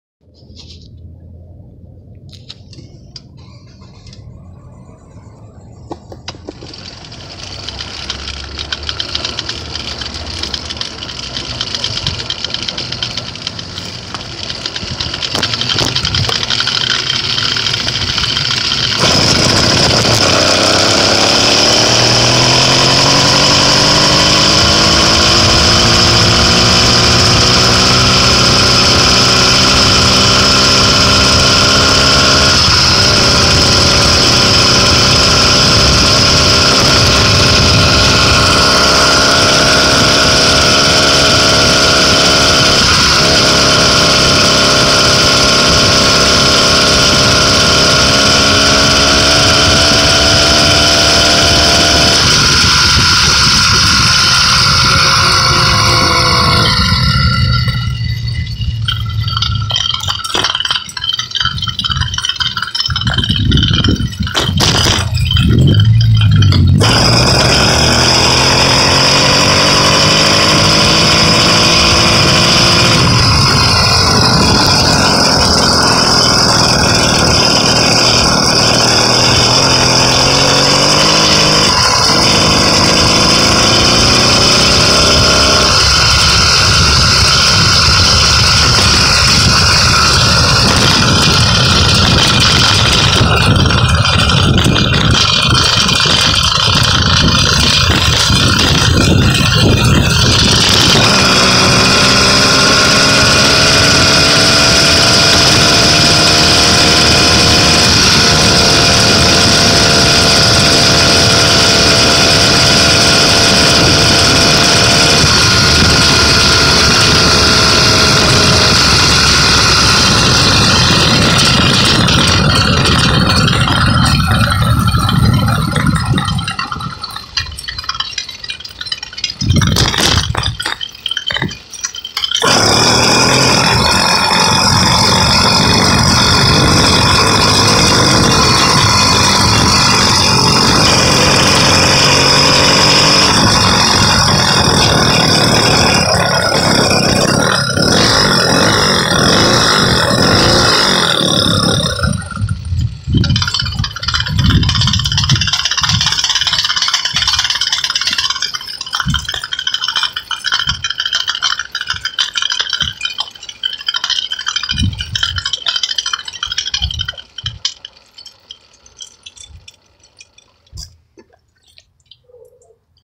Ya arregle la bicimoto
Unos amigos bicimoteros me ayudaron con sus consejos, despues note que la llanta estaba muy a la izquierda por eso topava a la llanta la cadena pero ya salimos a probarla y salio bien 🚴💨